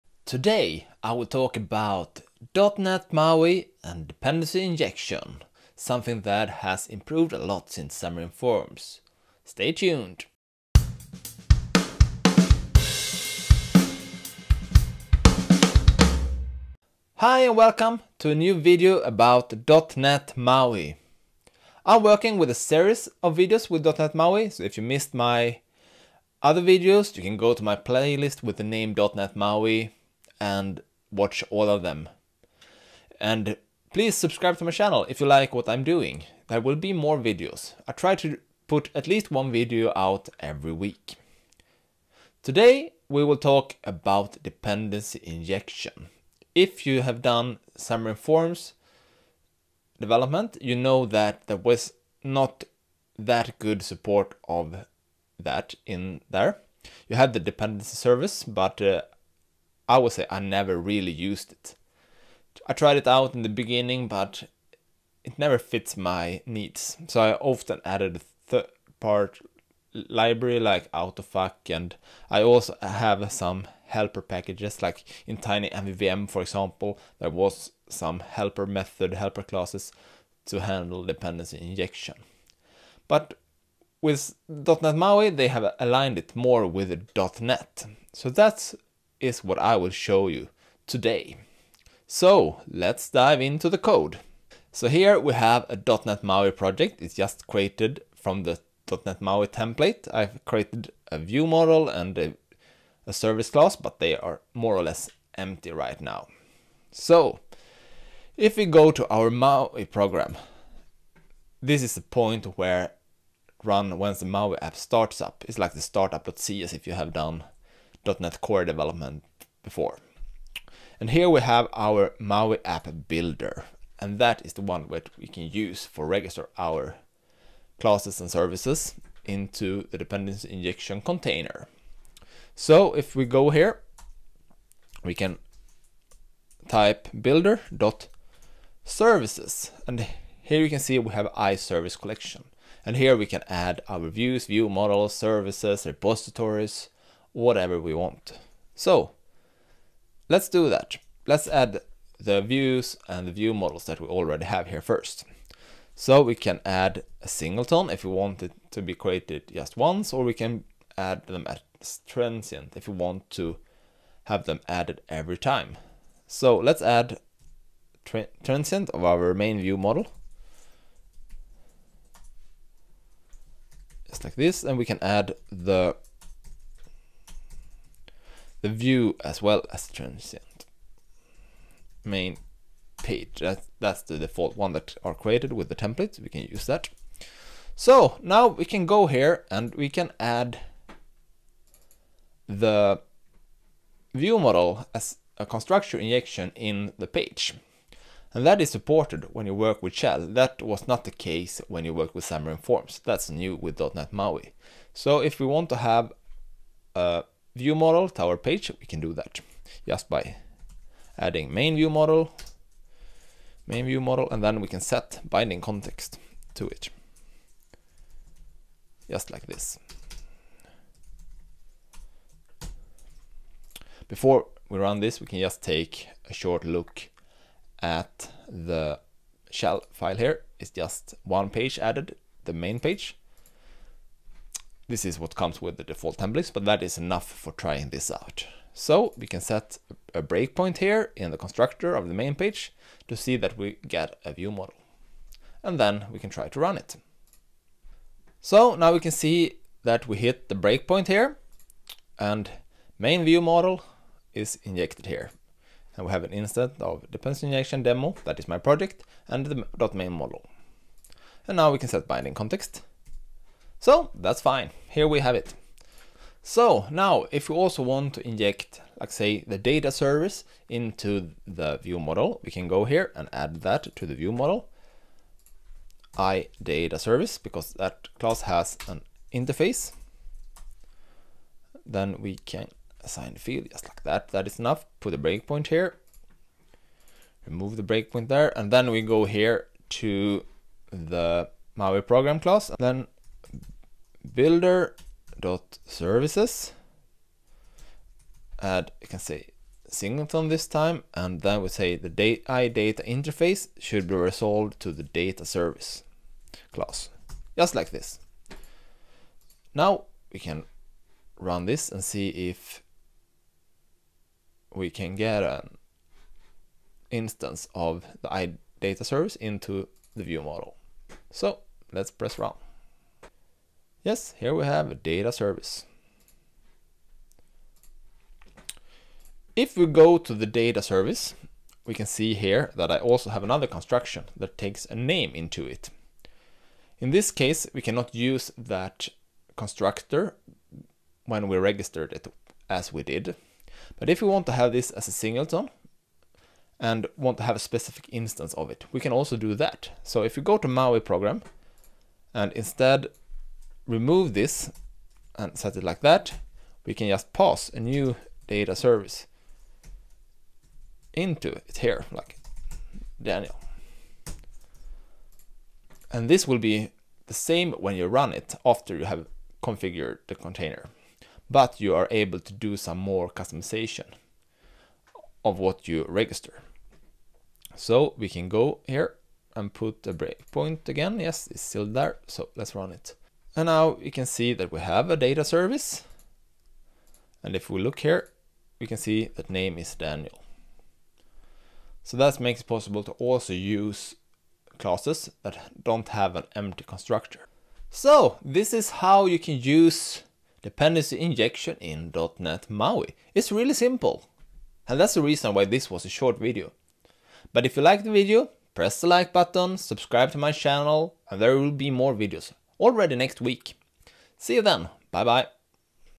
.NET MAUI and Dependency Injection
In this video, Daniel will show how to use Dependency Injection with .NET MAUI.